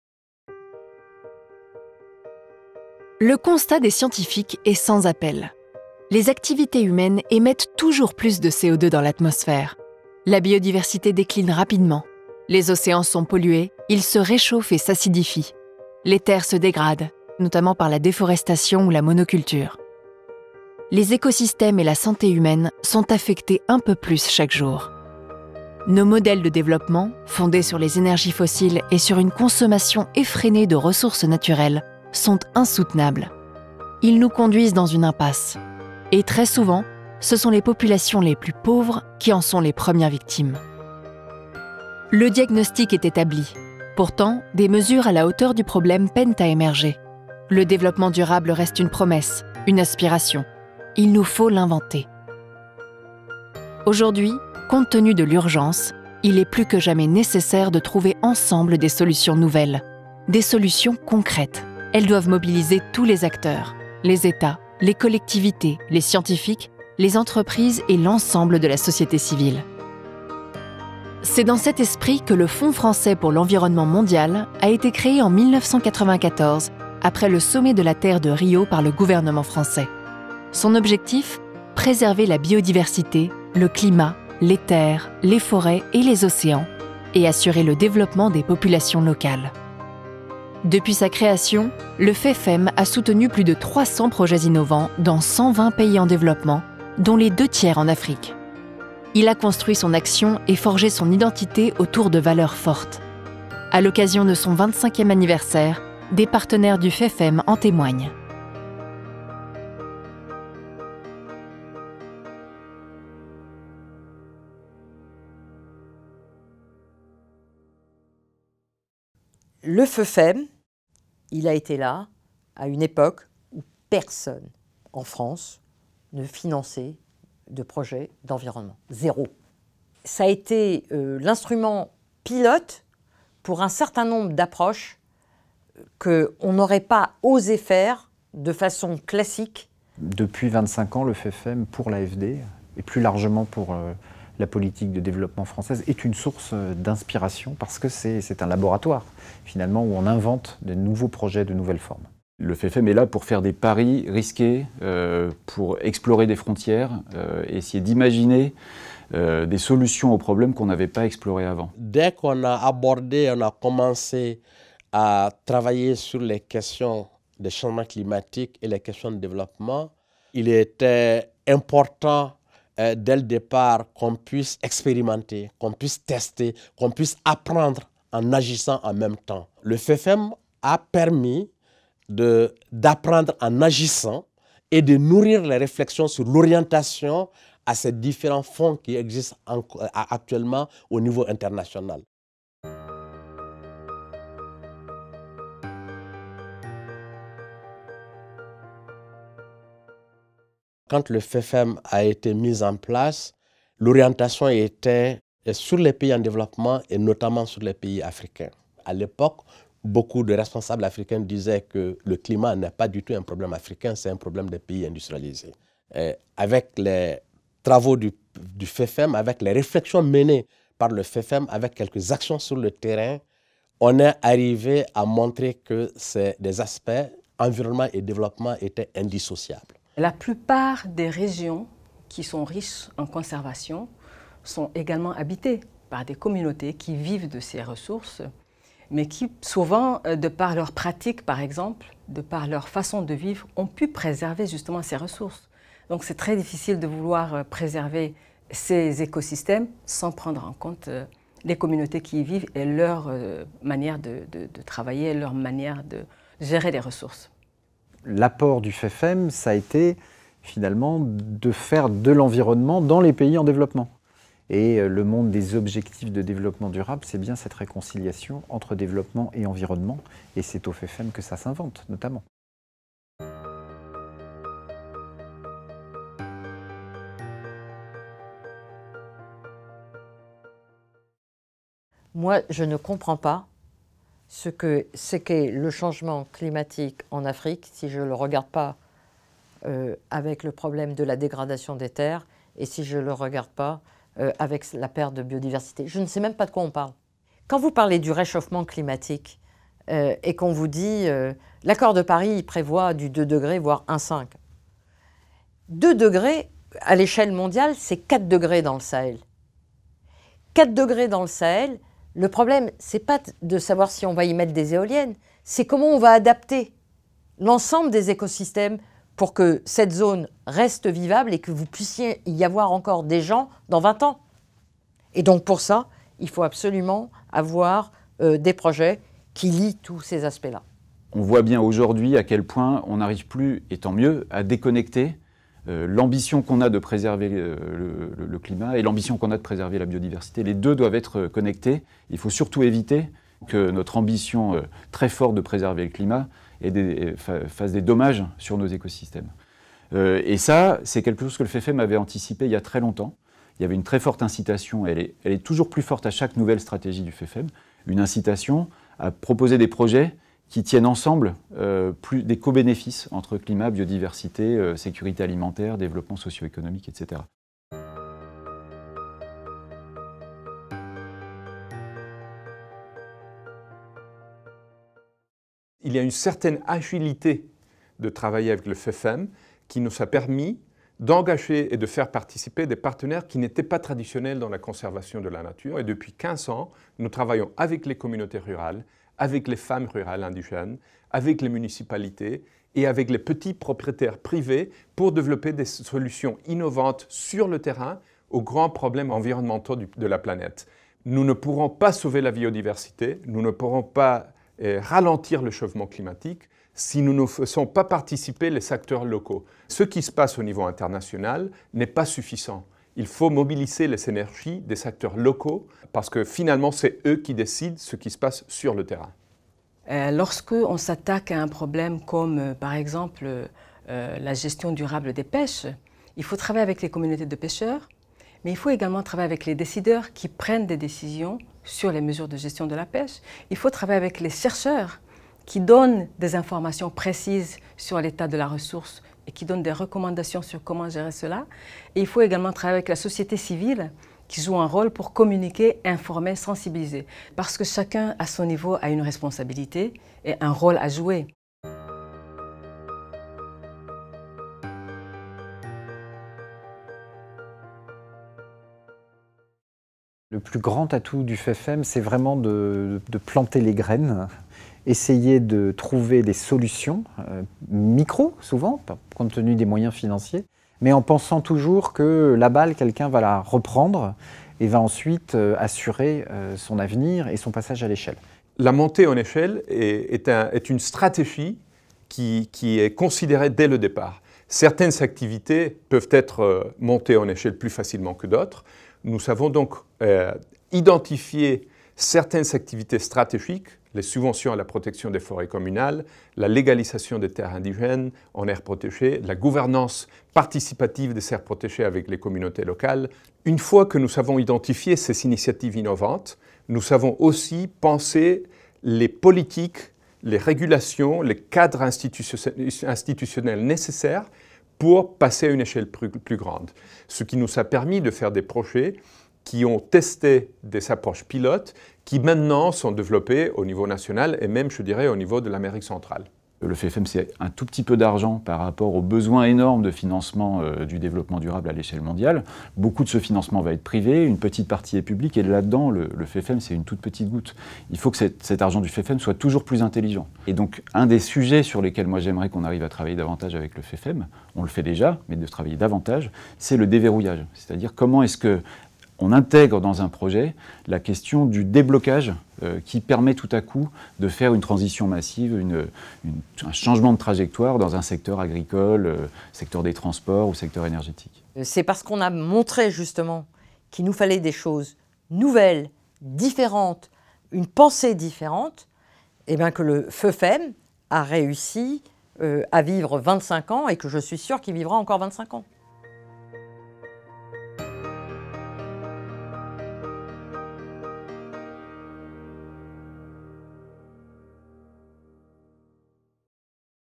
25 ANS DU FFEM (version longue) - français
Aujourd’hui, compte tenu de l’urgence environnementale, il est plus que jamais nécessaire de trouver des solutions nouvelles et concrètes.  Elles doivent mobiliser tous les acteurs : les états, les collectivités, les scientifiques, les entreprises et l’ensemble de la société civile.

C’est dans cet esprit que le Fonds Français pour l’Environnement Mondial  a  été créé en 1994 après le Sommet de la Terre de Rio par le gouvernement français. Son objectif : préserver la biodiversité, le climat, les terres, les forêts et les océans, et assurer le développement des populations locales.

Depuis sa création, le FFEM a soutenu plus de 300 projets innovants dans 120 pays en développement, dont les deux tiers en Afrique. Il a construit son action et forgé son identité autour de valeurs fortes.  A l’occasion de son 25ème anniversaire, des partenaires du FFEM en témoignent.